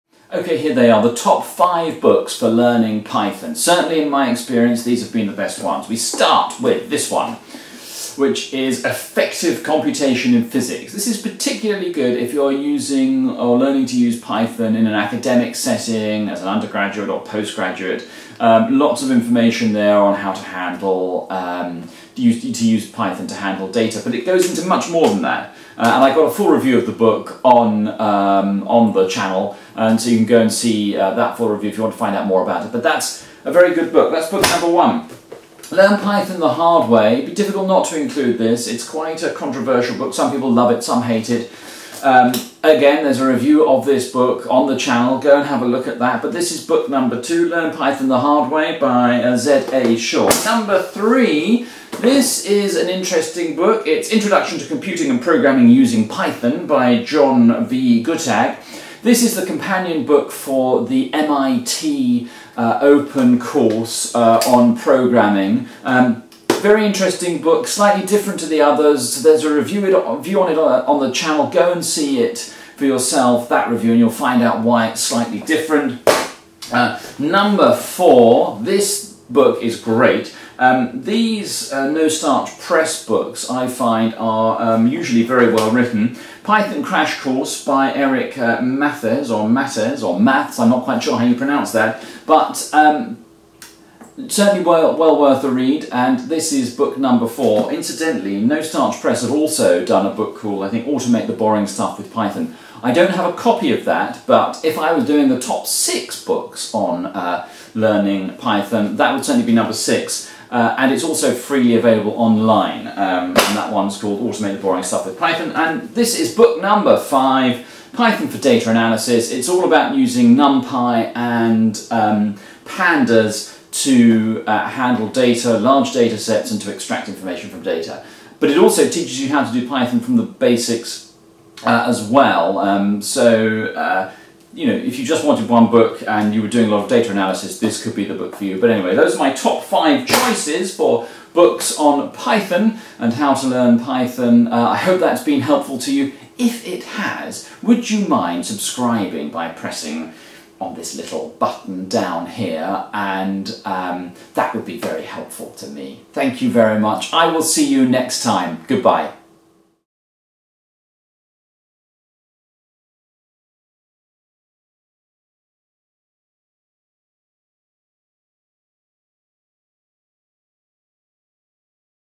Good books on python
Here is a short review of the top 5 best books for learning to program in Python. I think these are really good books on python and any of them will really help you to learn python from scratch. Whether you want to learn python for machine learning, data analysis, making games of for web app development, there will be something here for you. All of the books mentioned here have been reviewed separately on this channel.

1. Automate the Boring Stuff With Python
2. Python Crash Course
3. Effective Computation in Physics
4. Learn Python the Hard Way